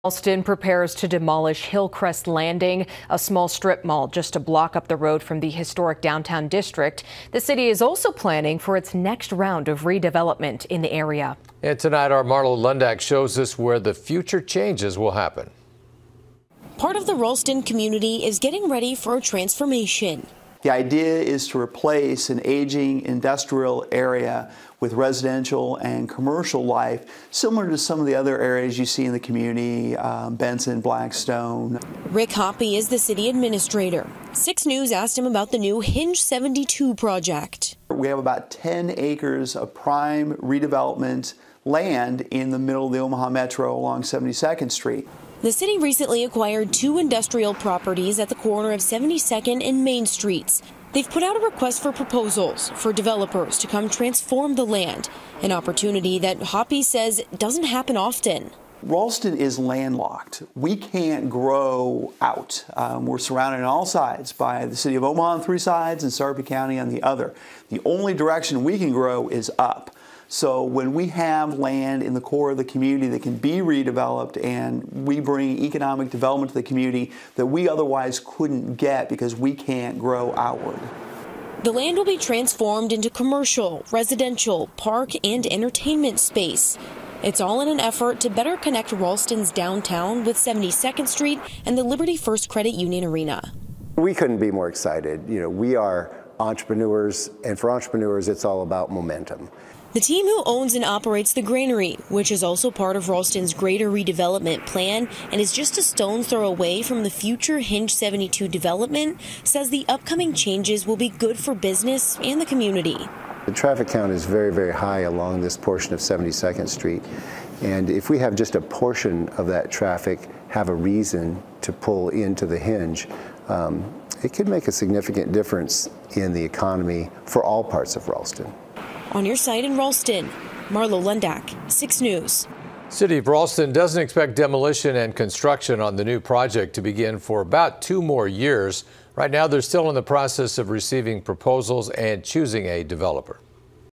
'Hinge 72' project creates new development in Ralston
The new Hinge 72 project creates opportunities for new developments in Ralston.